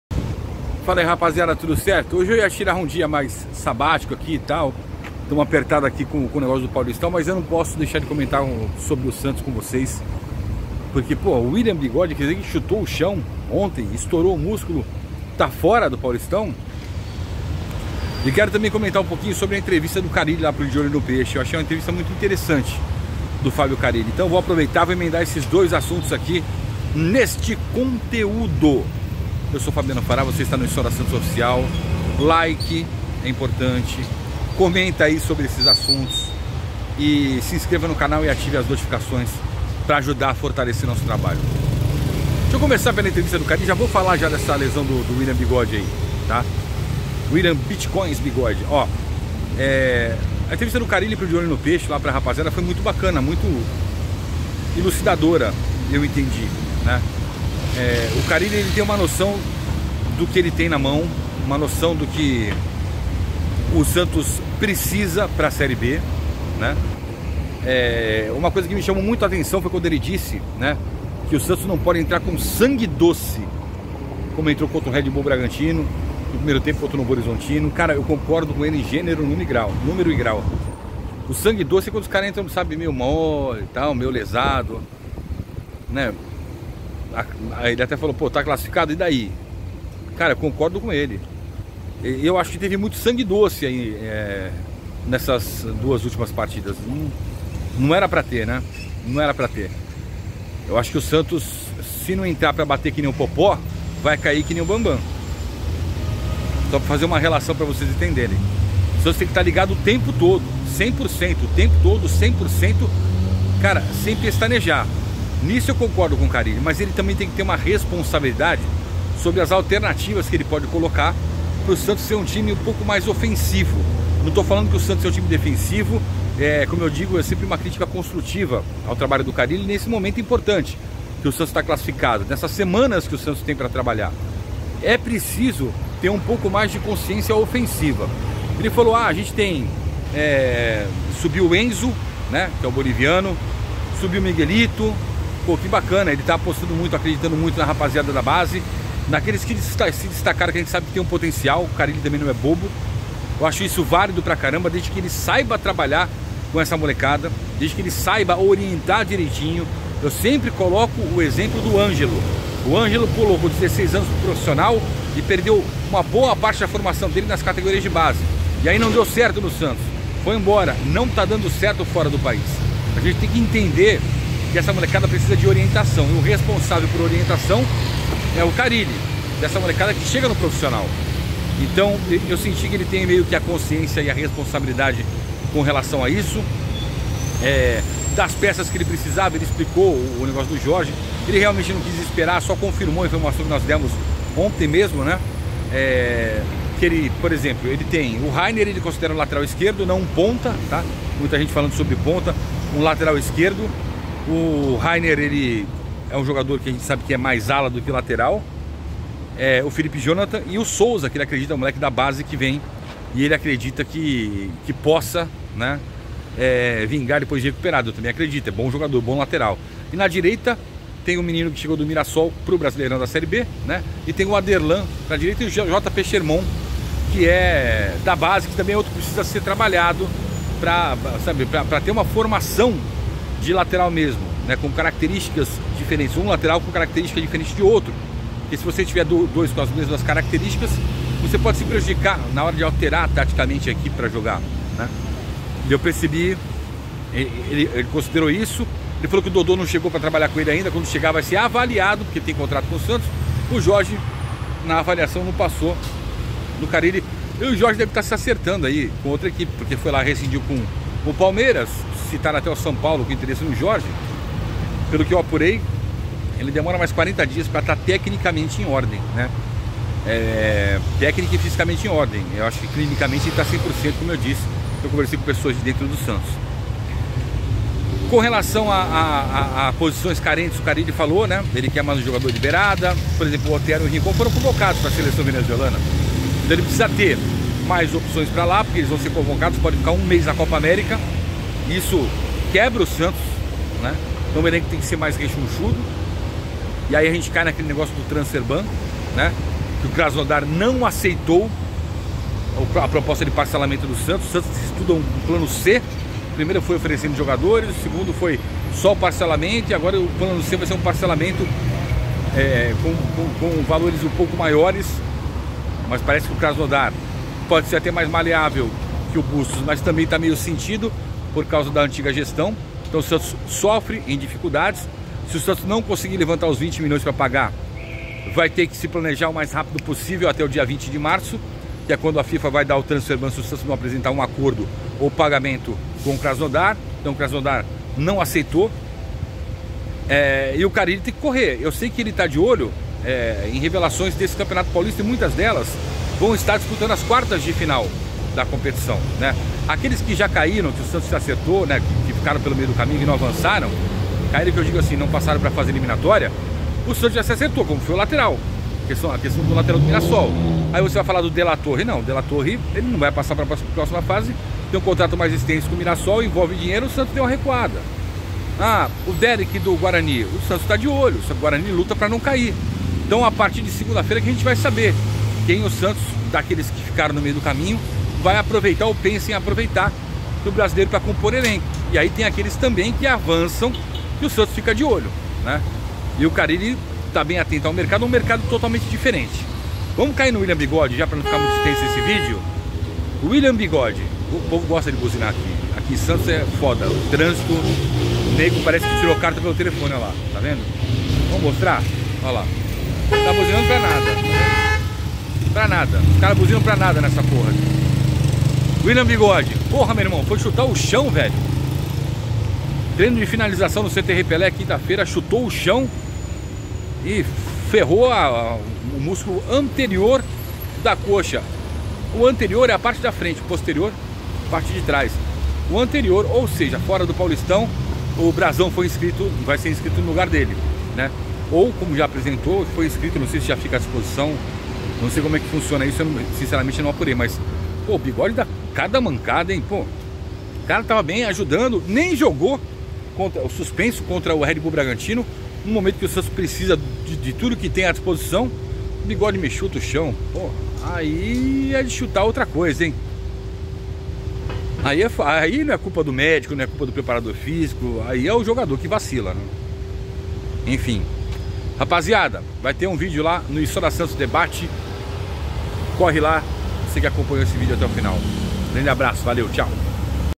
BIGODE FORA DO PAULISTÃO | CARILLE CONSCIENTE | KRASNODAR | ÚLTIMAS DO SANTOS!
NOVOS CANAIS

E SÓ DÁ SANTOS - OFICIAL

RADIO ESPORTE NEWS FM - TODOS OS GOLS DA RODADA DO BRASILEIRÃO